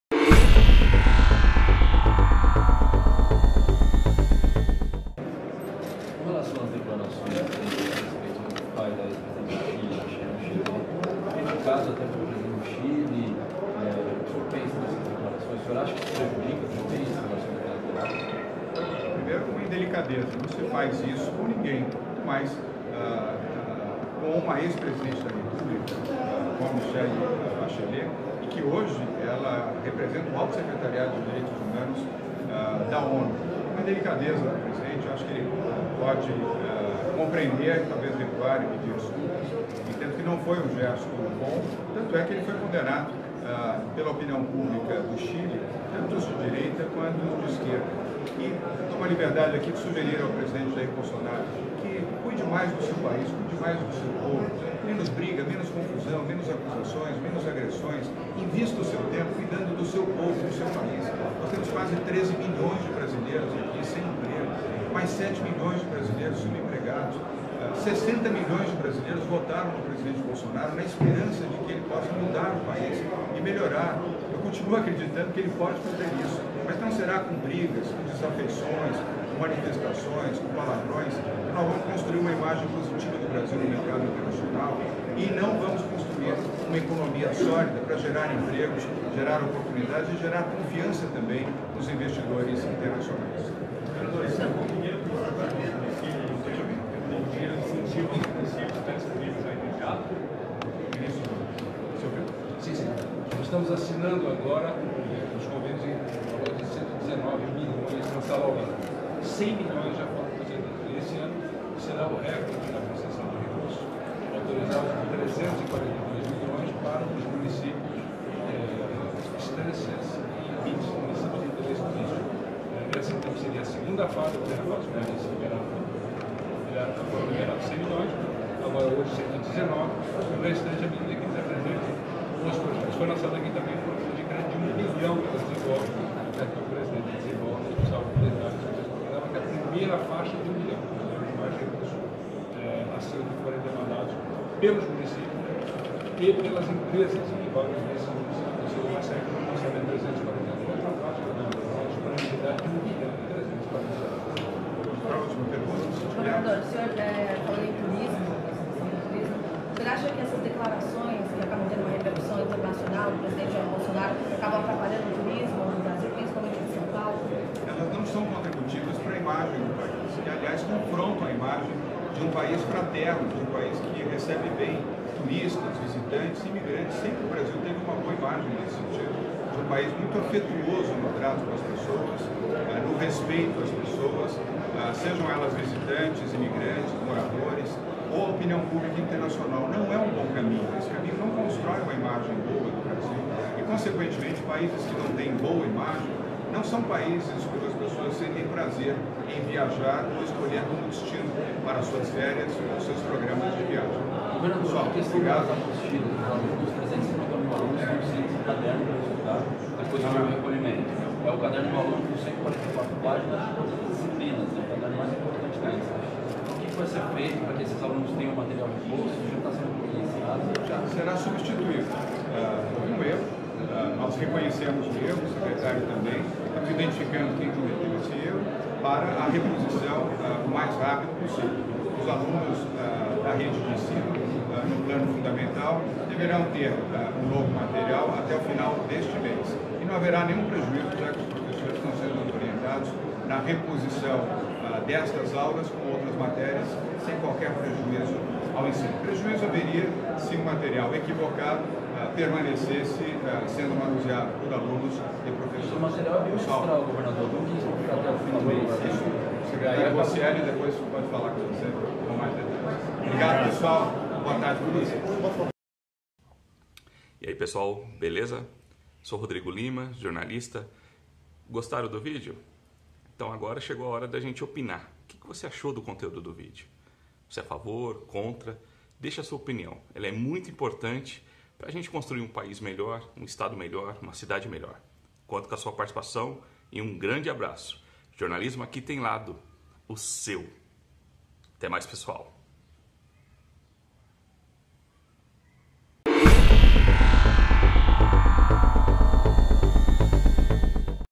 JOÃO DORIA MANDOU UM CONSELHO PARA BOLSONARO - Menos brigas, confusões, agressões e palavrões
O governador de São Paulo, João Doria (PSDB), mandou um conselho ao presidente Jair Bolsonaro (PSL) nesta quinta-feira, 5. O tucano pediu para o presidente governar. "Tomo aqui a liberdade de sugerir ao presidente Jair Bolsonaro, que cuide mais do seu país e do seu povo. Menos briga, menos confusão, menos acusações e menos agressões. Invista o seu tempo no seu povo. Temos quase 13 milhões  sem empregos", afirmou Doria.